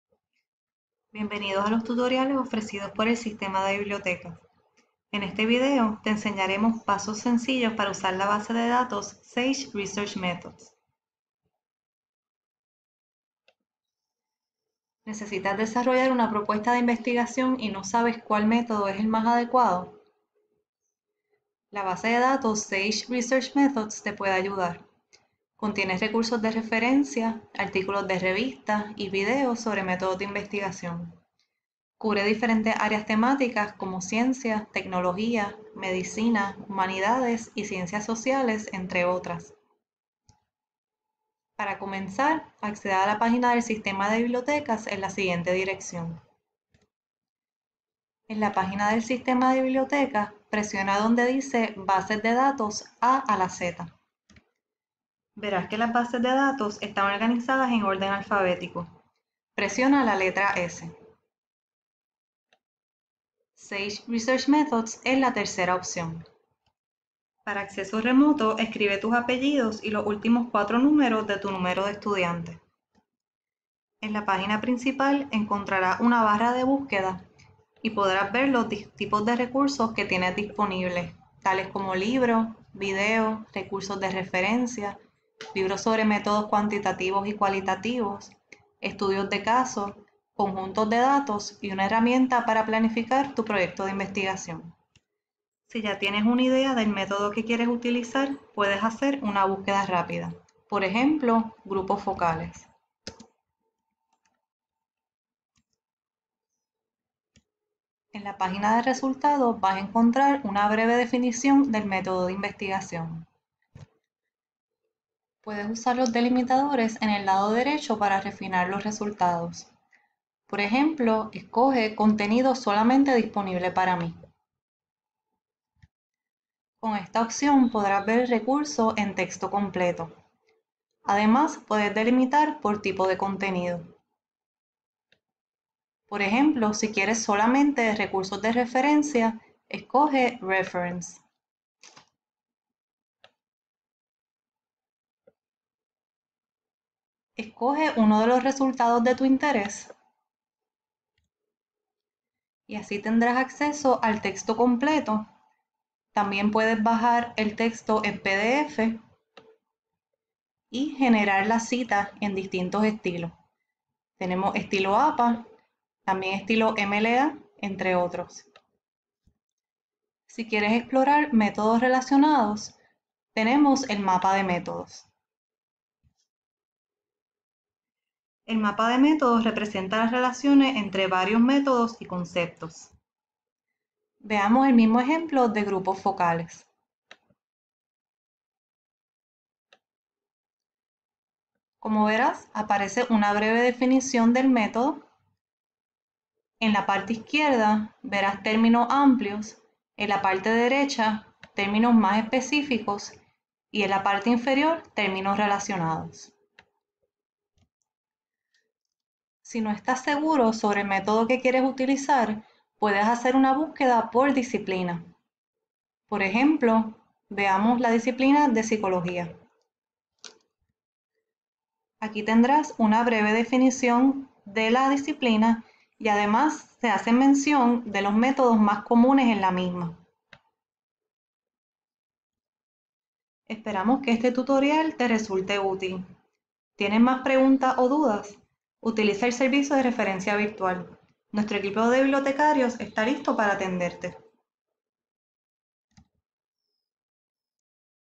Tutorial SageResearchMethods
El tutorial explica paso a paso cómo utilizar la base de datos Sage Research Methods, los diferentes contenidos de la misma así como las áreas temáticas que cubre.